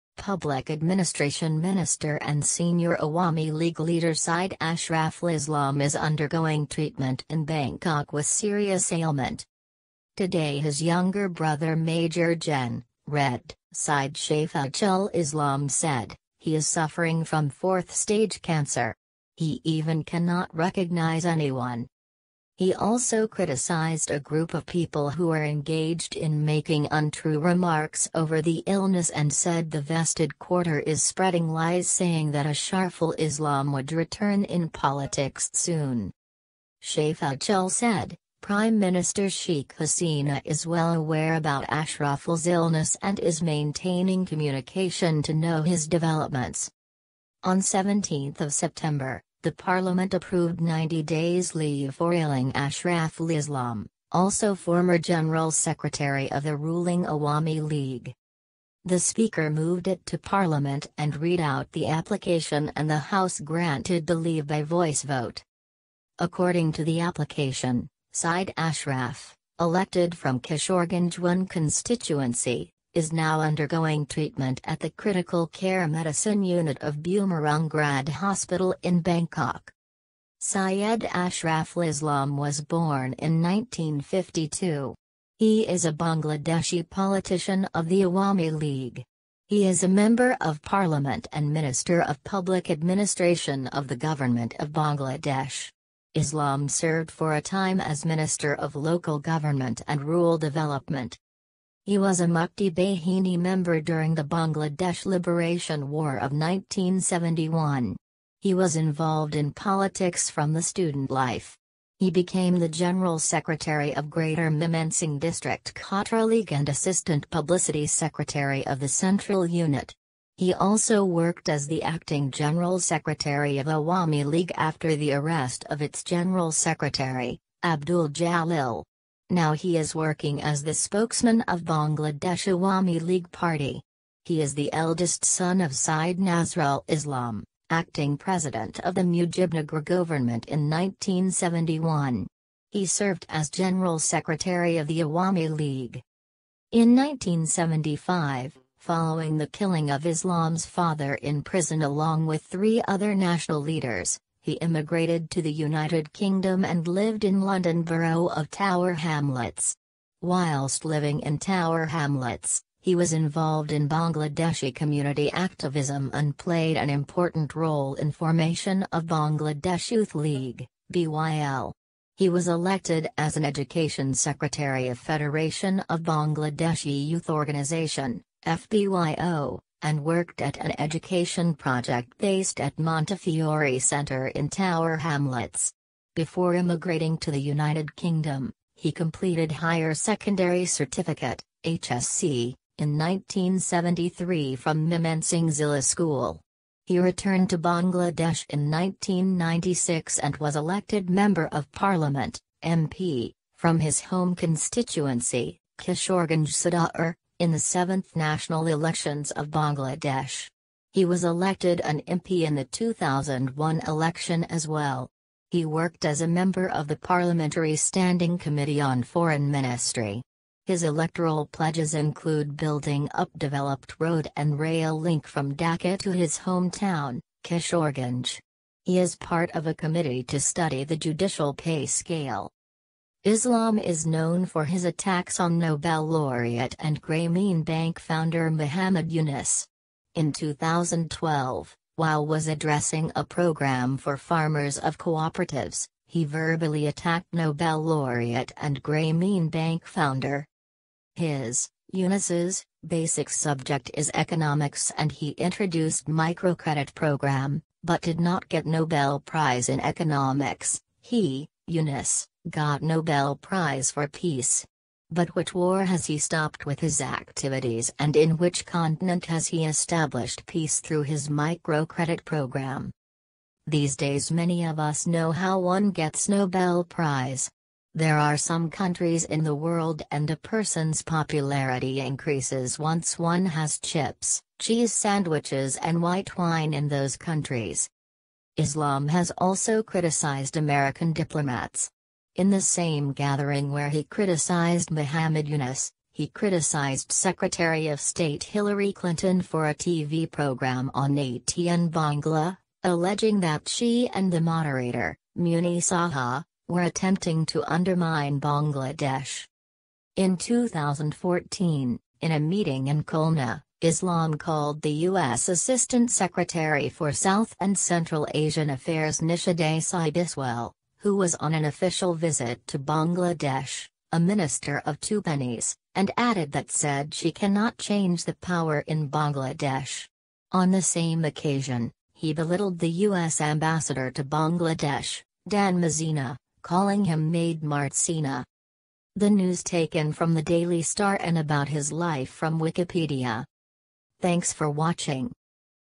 Life of Ashraful সৈয়দ আশরাফ আর নেই Cancer Treatment (Hard coded)
This video discusses the latest news and the life of a great leader. For better understanding and improving English the hard coded subtitles have also been added.

Public Administration Minister and senior Awami League leader Syed Ashraful Islam is undergoing treatment in Bangkok with serious ailment.
Today his younger brother Major Gen (retd) Syed Shafayetul Islam said, “He is suffering from fourth stage cancer. He even cannot recognise anyone,”
He also criticised a group of people who are engaged in making untrue remarks over the illness and said the vested quarter is spreading lies saying that Asharful Islam would return in politics soon.
Shafayetul said, Prime Minister Sheikh Hasina is well-aware about Ashraful’s illness and is maintaining communication to know his developments,
On 17 September, the parliament approved 90 days' leave for ailing Ashraful Islam, also former general secretary of the ruling Awami League.
The Speaker moved it to parliament and read out the application and the House granted the leave by voice vote.
According to the application, Syed Ashraf, elected from Kishoreganj-1 constituency, is now undergoing treatment at the Critical Care Medicine Unit of Bumrungrad Hospital in Bangkok.
Sayed Ashraful Islam was born in 1952. He is a Bangladeshi politician of the Awami League. He is a Member of Parliament and Minister of Public Administration of the Government of Bangladesh. Islam served for a time as Minister of Local Government and Rural Development.
He was a Mukti Bahini member during the Bangladesh Liberation War of 1971. He was involved in politics from the student life. He became the General Secretary of greater Mymensingh District Chhatra League and assistant publicity secretary of the central unit. He also worked as the Acting General Secretary of Awami League after the arrest of its general secretary, Abdul Jalil. Now he is working as the spokesman of Bangladesh Awami League Party. He is the eldest son of Syed Nazrul Islam, acting president of the Mujibnagargovernment in 1971.  He served as General Secretary of the Awami League.
In 1975, following the killing of Islam's father in prison along with three other national leaders, he immigrated to the United Kingdom and lived in London Borough of Tower Hamlets. Whilst living in Tower Hamlets, he was involved in Bangladeshi community activism and played an important role in formation of Bangladesh Youth League (BYL). He was elected as an Education Secretary of Federation of Bangladeshi Youth Organisation (FBYO) and worked at an education project based at Montefiore Centre in Tower Hamlets. Before immigrating to the United Kingdom, he completed Higher Secondary Certificate (HSC) in 1973 from Mymensingh Zillah School. He returned to Bangladesh in 1996 and was elected Member of Parliament (MP) from his home constituency, Kishoreganj Sadar, in the 7th national elections of Bangladesh. He was elected an MP in the 2001 election as well. He worked as a member of the parliamentary standing committee on foreign ministry. His electoral pledges include building up developed road and rail link from Dhaka to his home town, Kishoreganj. He is part of a committee to study the judicial pay scale.
Islam is known for his attacks on Nobel laureate and Grameen Bank founder Muhammad Yunus. In 2012, while was addressing a program for farmers of cooperatives, he verbally attacked Nobel laureate and Grameen Bank founder:
His (Yunus's) basic subject is economics and he introduced microcredit programme, but did not get Nobel Prize in economics, ... He (Yunus) got Nobel Prize for Peace. But which war has he stopped with his activities and in which continent has he established peace through his microcredit program?
These days many of us know how one gets Nobel Prize. There are some countries in the world and a person's popularity increases once one has chips, cheese sandwiches and white wine in those countries.
Islam has also criticised American diplomats. In the same gathering where he criticised Muhammad Yunus, he criticised Secretary of State Hillary Clinton for a TV program on ATN Bangla, alleging that she and the moderator, Munni Saha, were "attempting to undermine Bangladesh”.
In 2014, in a meeting in Khulna, Islam called the US Assistant Secretary for South and Central Asian Affairs Nisha Desai Biswal, who was on an official visit to Bangladesh, "a minister of two pennies," and added that said she "cannot change the power in Bangladesh”. On the same occasion, he belittled the US Ambassador to Bangladesh, Dan Mozena, calling him "maid Marzina”.

[The news taken from The Daily Star & about his life from Wikipedia]